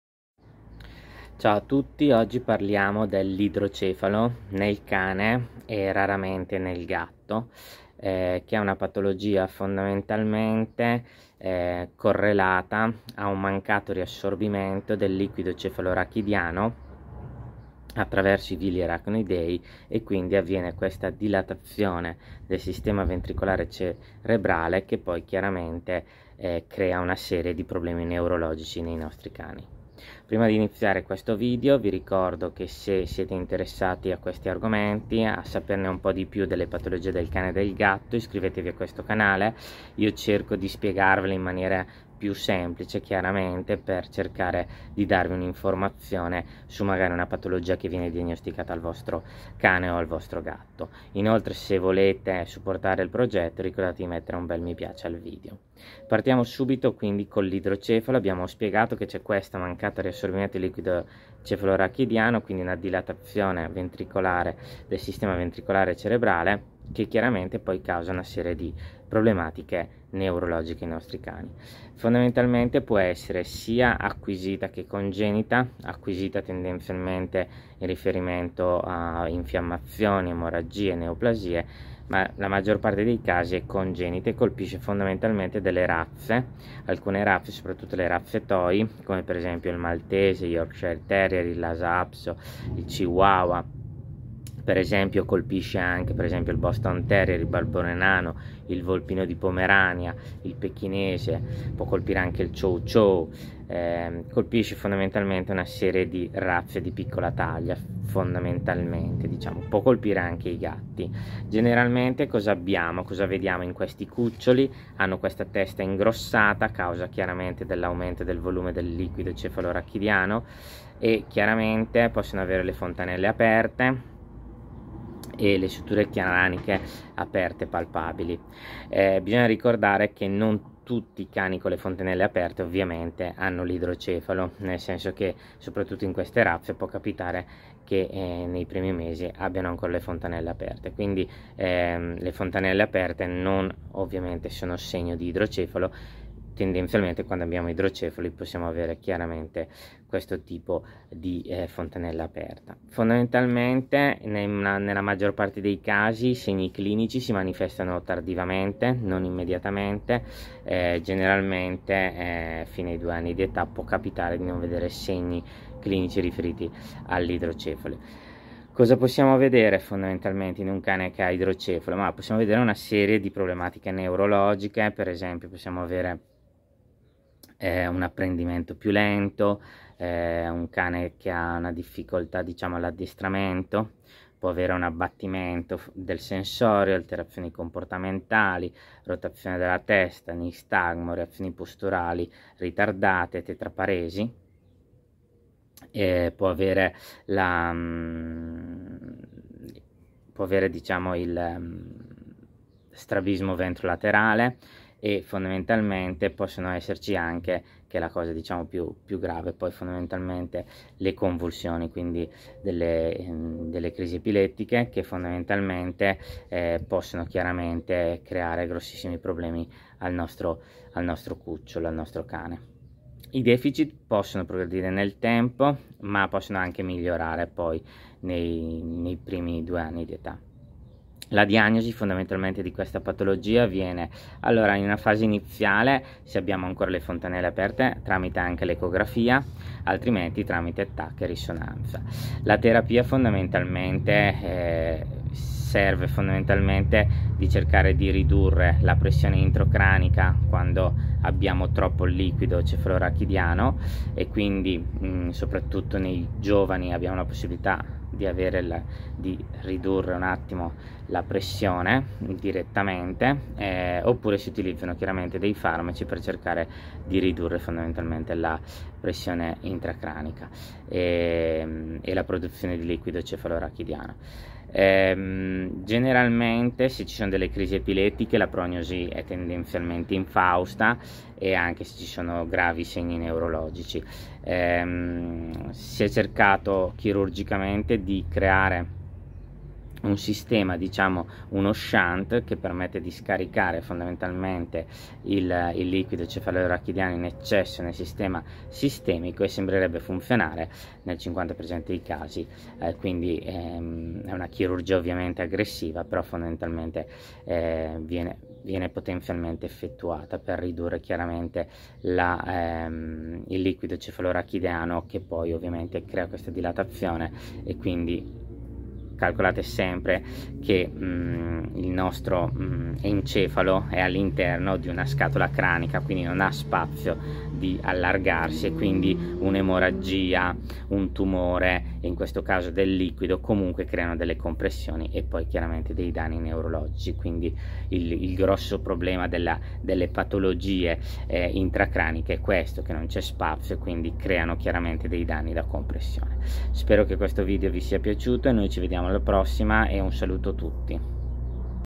Idrocefalo nel cane e nel gatto
Nell'idrocefalo il sistema ventricolare cerebrale si dilata a causa di un'ostruzione al flusso del liquido cefalorachidiano che non viene riassorbito dal circolo sistemico attraverso i villi aracnoidei. Colpisce le razze toy come Maltese, Chihuahua, Yorkshire terrier, Lhasa Apso, Volpino di Pomerania, Barbone nano, Carlino, Pechinese ma anche razze come Bulldog inglese, Chow chow.